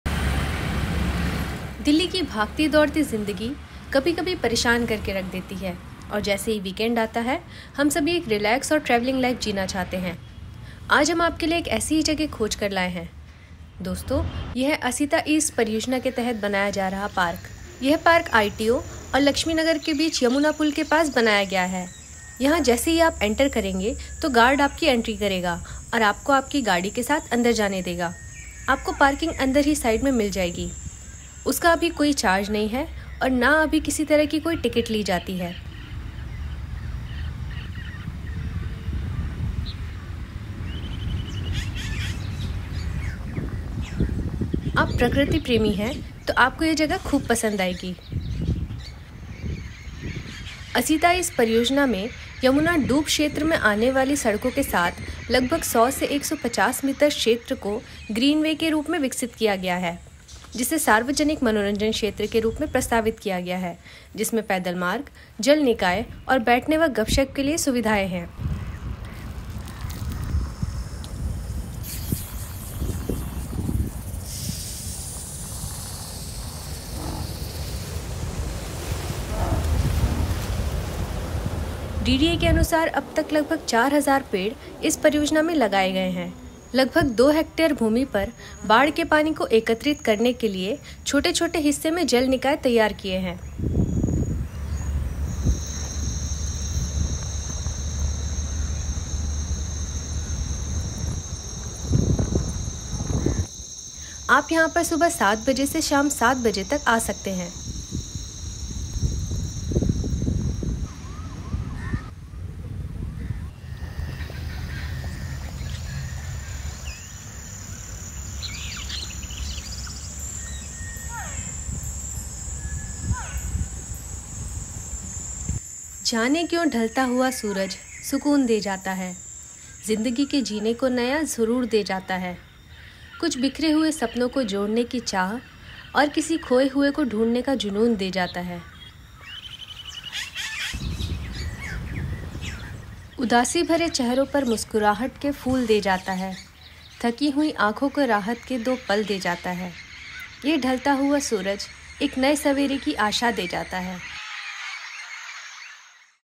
Asita East, River Yamuna | ताजा हवा और सुकून भरा Delhi tourist places | DDA | असिता ईस्ट परियोजना
दिल्ली की दौड़ती भागती जिंदगी आपको परेशान कर देती है।  आज हम आपके लिए ऐसी ही एक जगह खोज कर लाएं हैं। दोस्तों ये हैं असिता ईस्ट परियोजना के तहत बनाया जा रहा पार्क। यह पार्क ITO और laxmi नगर के बीच यमुना पुल के पास बनाया गया है। यहाँ जैसे ही आप इंटर करेंगे तो gurad आपकी एंट्री करेगा और आपको अपनी गाड़ी के साथ अंदर जाने देगा। आपको पार्किंग अंदर ही साइड में मिल जाएगी उसका अभी कोई चार्ज नहीं है न ही अभी किसी तरह की कोई टिकट ली जाती हैं. आप प्रकृति प्रेमी हैं तो यह जगह आपको खूब पसंद आएंगी।

Asita East project on River Yamuna | Delhi Development Authority (DDA)